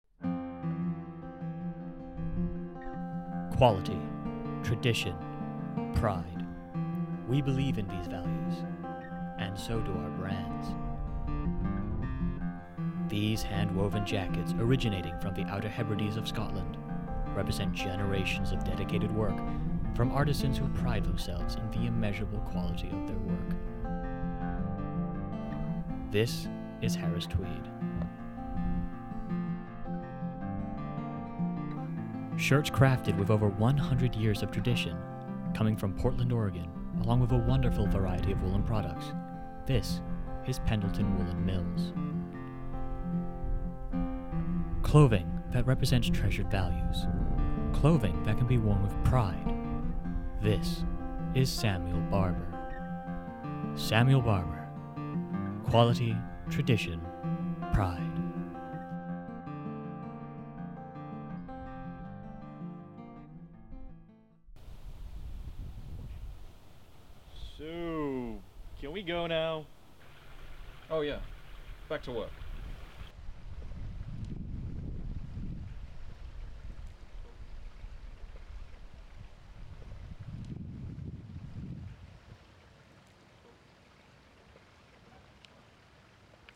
Samuel Barber Clothing - "Quality. Tradition. Pride"
The first commercial for a new clothing retailer: Samuel Barber Clothing.

Director: Corey Bulloch
Models: Anton Kudryashov Jr., Cal Madden, and Samuel Barber